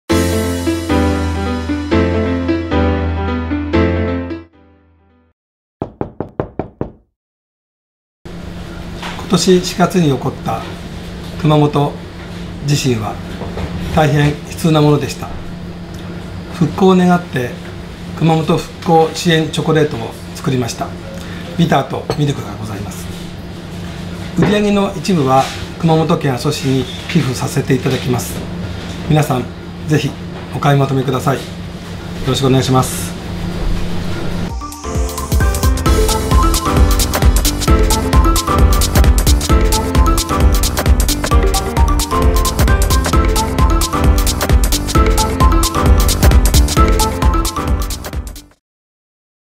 【SDC2017】テオブロマ 新作ショコラ紹介
ミュゼ・デュ・ショコラ テオブロマの土屋公二氏による新作ショコラ紹介。熊本復興のためのタブレット。